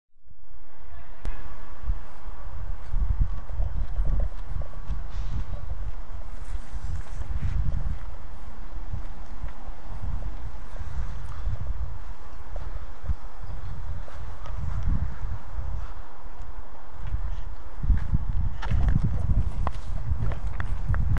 Upper Haight St. Mural Classic Rocker's Style
Who can read the text message written within the globe @ 0.02? Here it is (backwards): ECAEP
WONK LLIW DLROW THE
,REWOP FO EVOL THE SEMOCREVO
,EVOL FO REWOP EHT NEHW